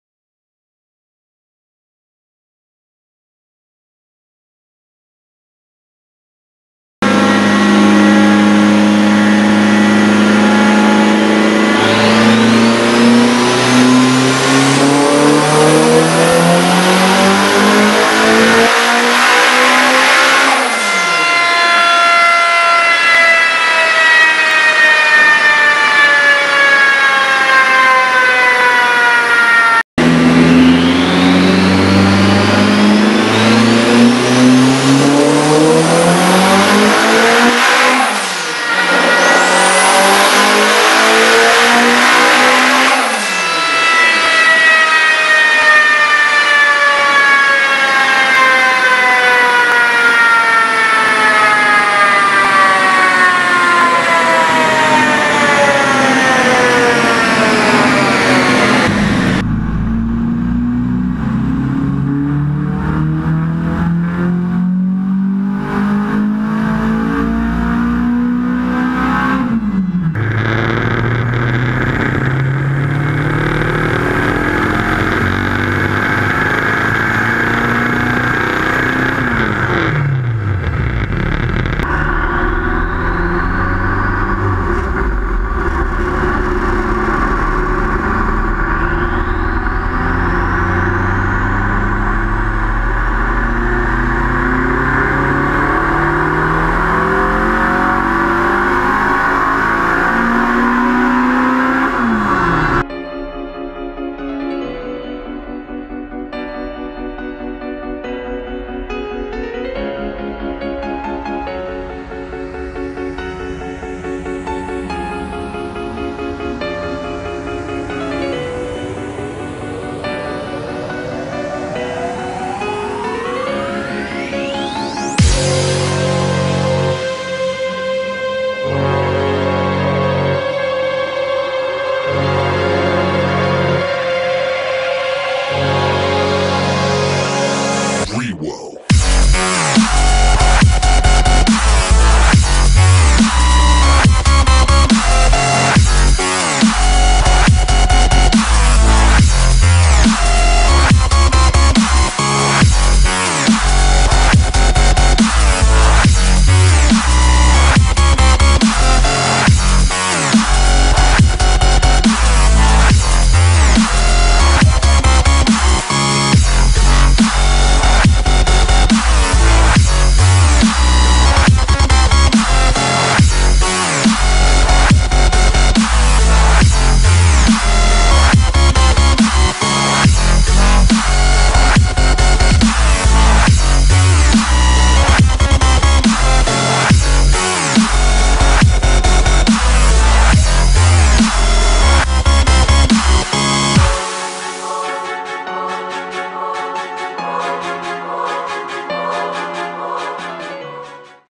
Turbo Civic dyno long version
999 Fine Racing Time Attack 1998 EK Civic on the dyno B18 Turbo Civic, FP Black Turbo, 2200cc injectors, E85, 10.5:1 compression, 22 LB boost